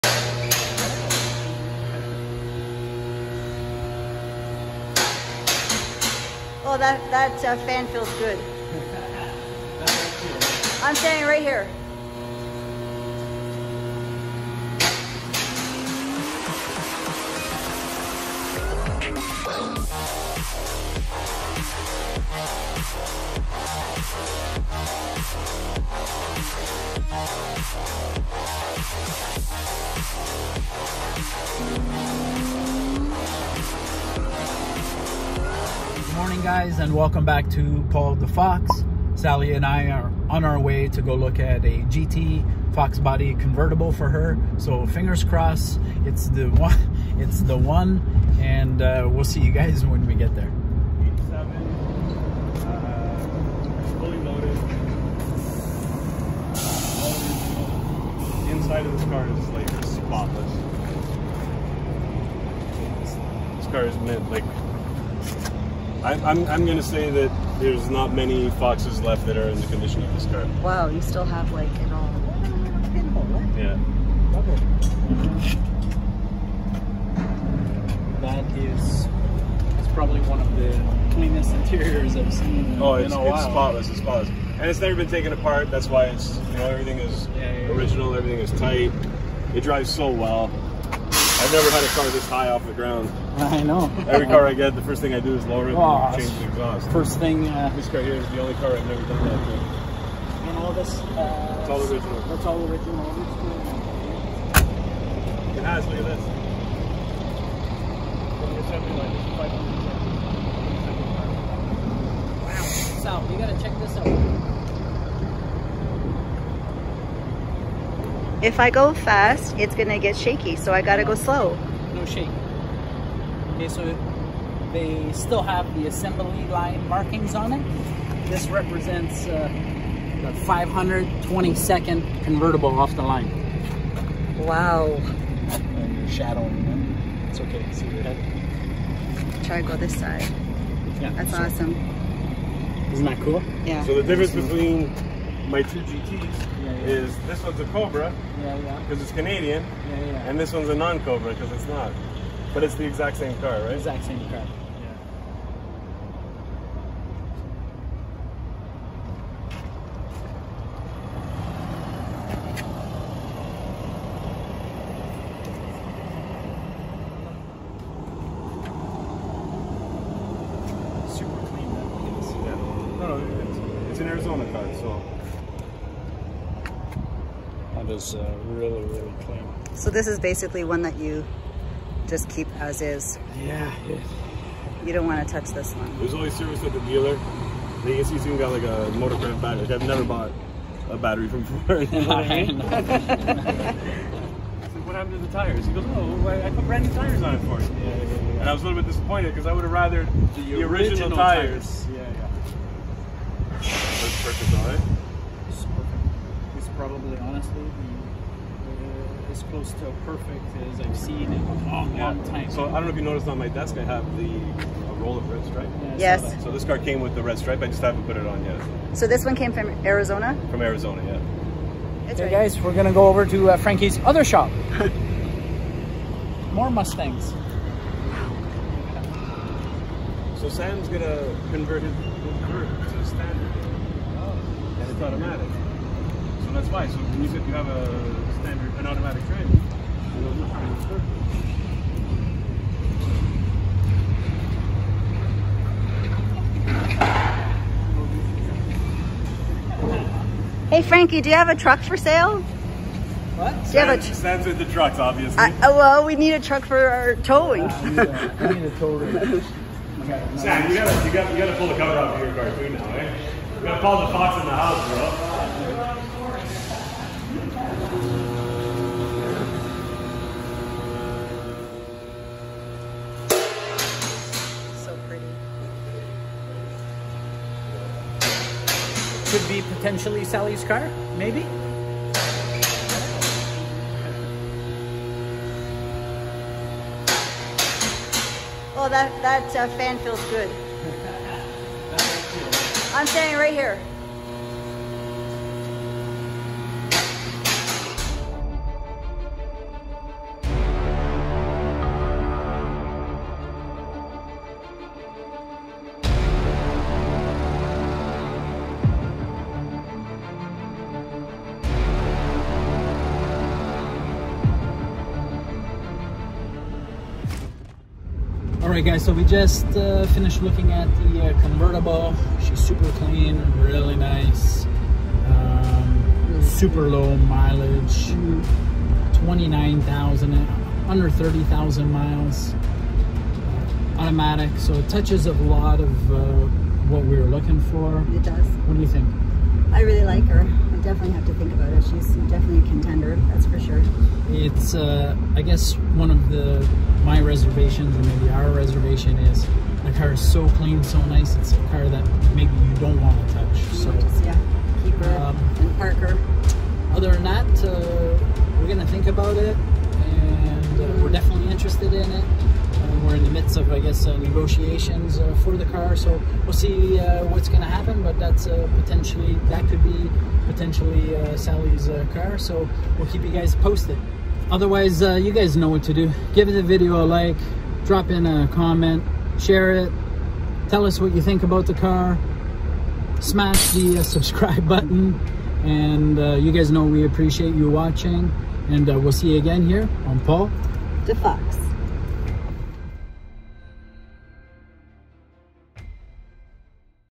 MINT FOXBODY MUSTANG GT CONVERTIBLE || Is This Sally's New Vert Fox?
Shopping for Sally's Foxbody Mustang Convertible - Check out this mint GT Vert!!

Paul DaFox 🦊